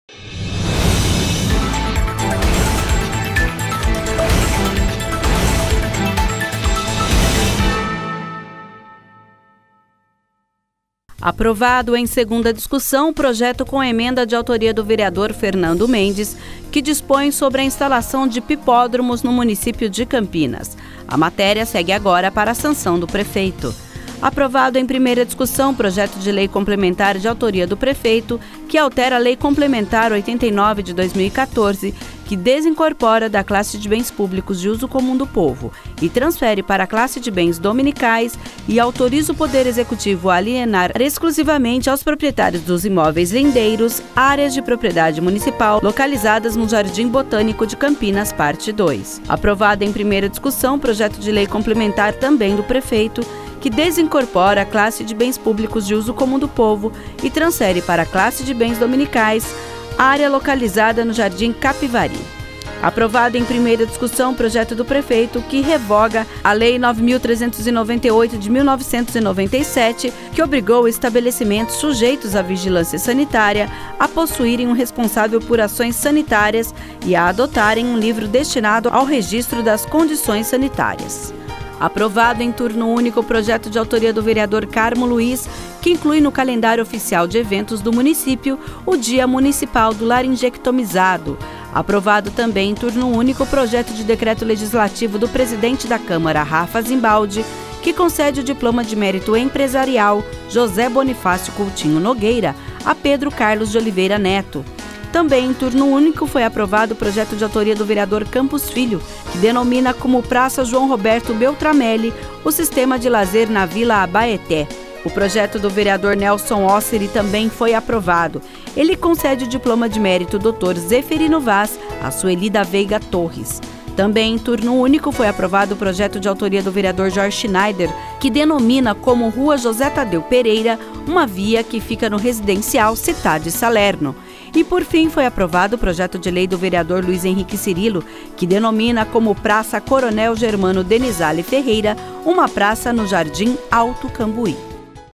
Câmara Notícia : Resultado 45ª Reunião Ordinária 16/085/2018
Veja o resultado da votação dos projetos da pauta da 45ª Reunião Ordinária.